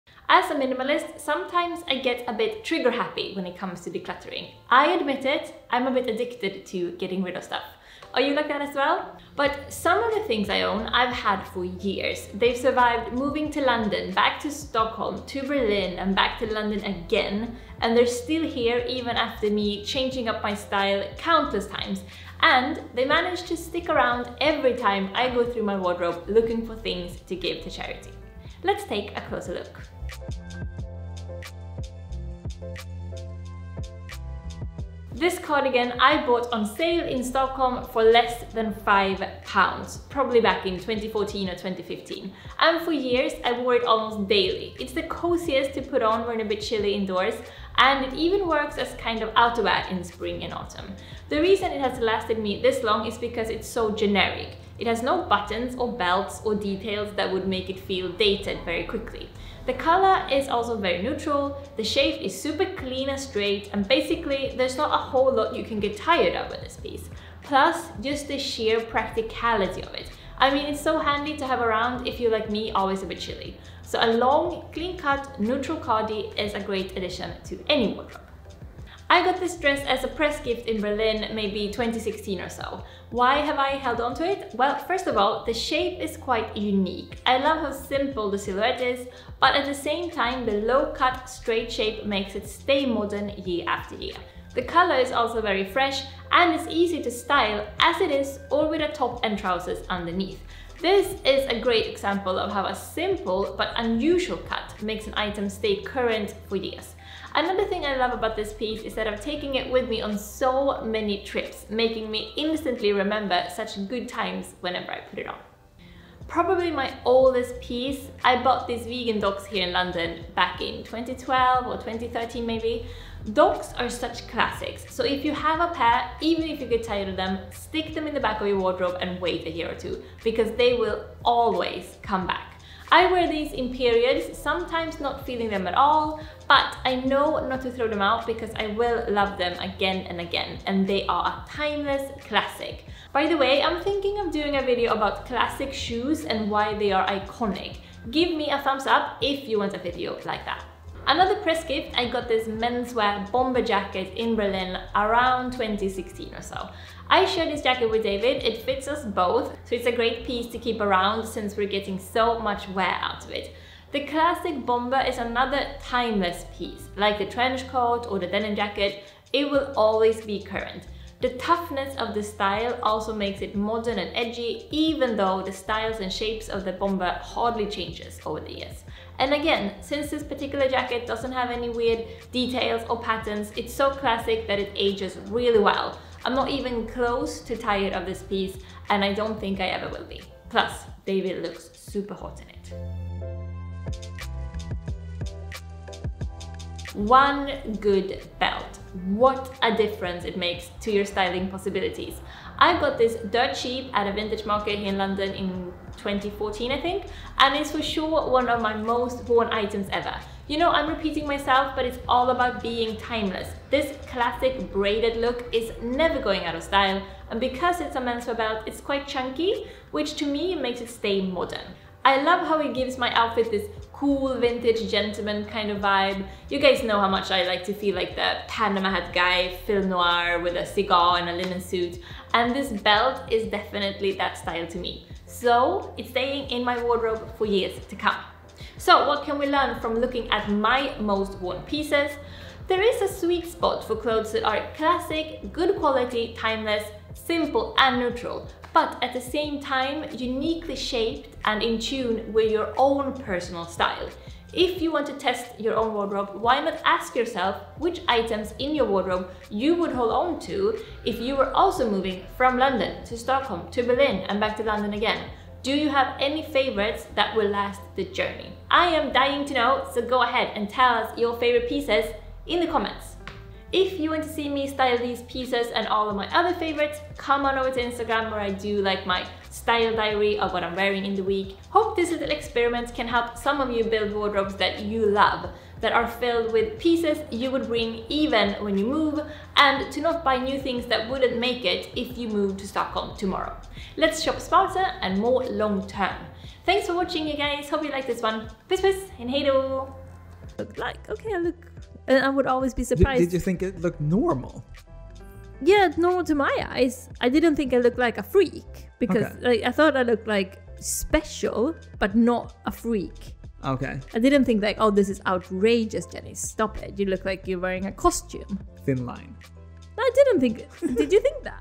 STOP DECLUTTERING ! – 5 things you should keep
i admit it, i might have a minimalist addiction to clearing out my wardrobe. but still, there are some items that seem to survive every decluttering session: the items you SHOULD NOT get rid of !
here are some of my favourite pieces that i’ve kept for aaages and that have survived moving from london to stockholm, to berlin and back to london again !

what are your favourite items that you would never get rid of ?

i talk about:
- decluttering your wardrobe !
- addicted to getting rid of stuff ?
- clothes that you should NOT get rid of !
- my favourite pieces !
- from london to stockholm to berlin and back to london !
- the perfect cardigan !
- generic, neutral, simple, clean-cut ?
- how to style uniquely shaped dresses !
- clothes with memories attached to them !
- pieces that stay current and modern for seasons and years ?
- vegan dr. martens !
- timeless and iconic shoes !
- wearing menswear !
- modern and edgy with the bomber !
- vintage / thrift shopping !
- never underestimate the perfect belt !
- vintage gentleman vibe ?

i am wearing:
- prada sunnies (pre-owned)
- monki cardigan
- monki dress (gifted)
- lee trousers (pre-owned)